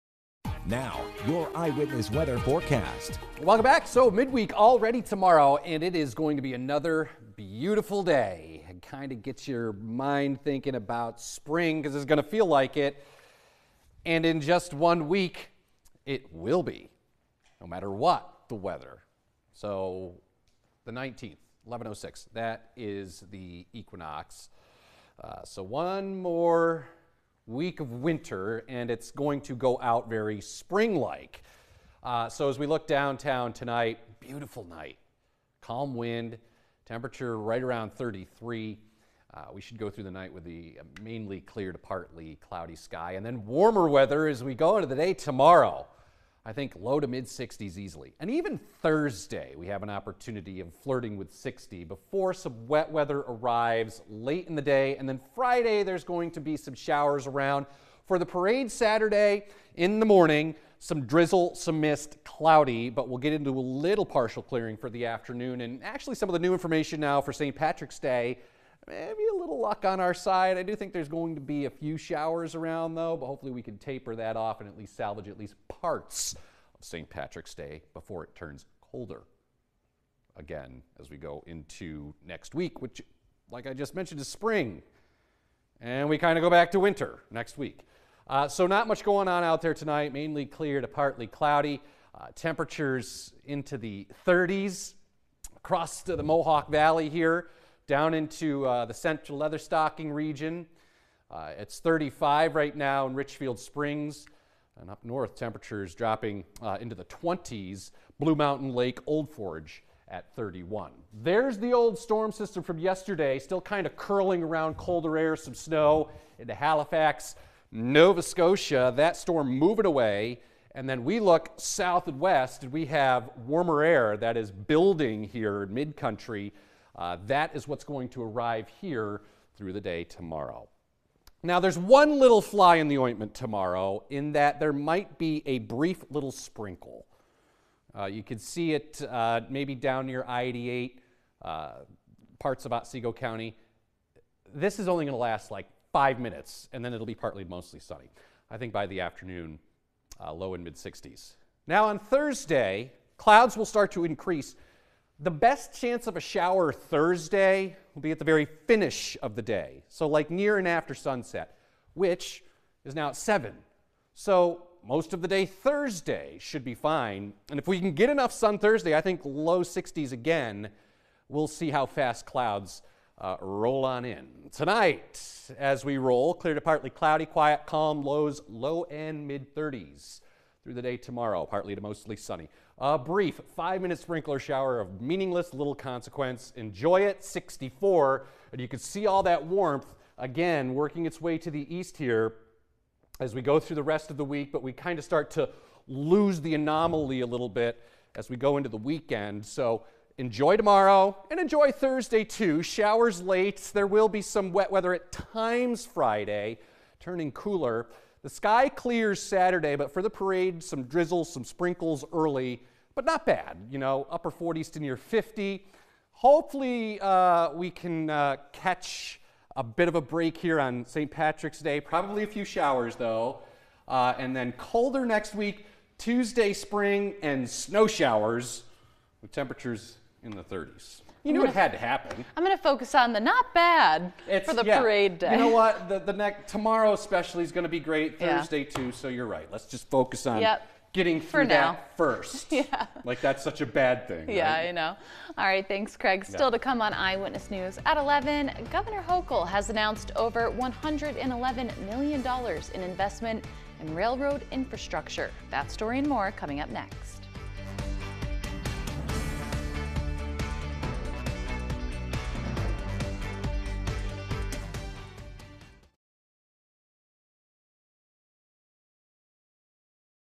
Eyewitness Weather 3-12-24
Meteorologist Craig Flint takes a look at the weather, including a big warm-up coming soon!